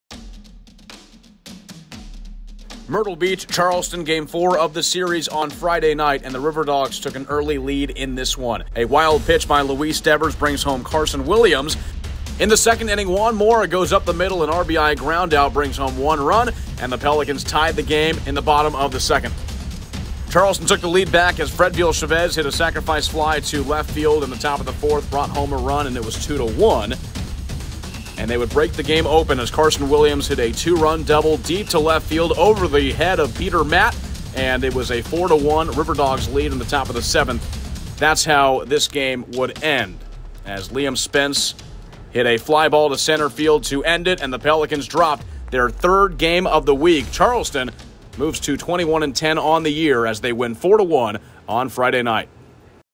Game Recap 5-13-2022
The Pelicans and Riverdogs met for the 4th game of the series on Friday Night. Here is all the action.